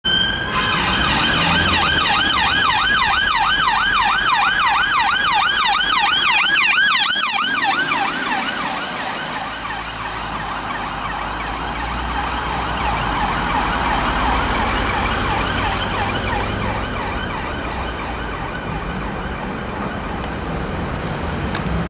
Greater Manchester Police BMW X5 ARV Responding On Blue Lights & Sirens
Greater Manchester Police (GMP) BMW X5 ARV (Armed Response Vehicle) responding near Thomson Street on blues and wailers going to an unknown job yesterday (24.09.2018)